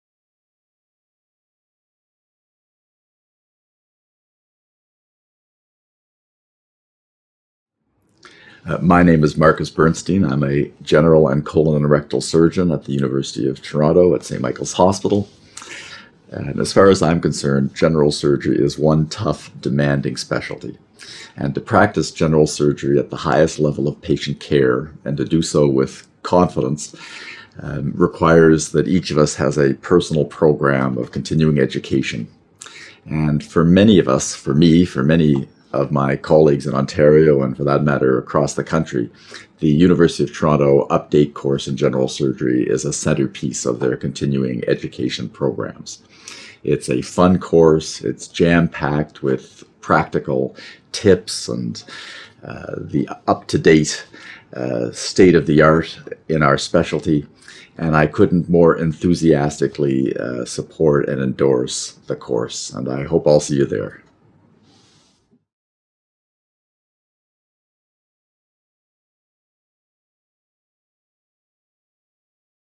Dr. Marcus Burnstein recommends the Update in General Surgery 2014
Marcus Burnstein, MD, FRCSC, Colon and Rectal Surgeon at St. Michaels Hospital, Department of Surgery in the Faculty of Medicine, University of Toronto, recommends this year's event at the Hilton Toronto Downtown on April 10-12, 2014. Please tweet any questions for the panel discussions with the official hashtag - UGS2014.

This video has been produced by Continuing Professional Development, Faculty of Medicine, University of Toronto.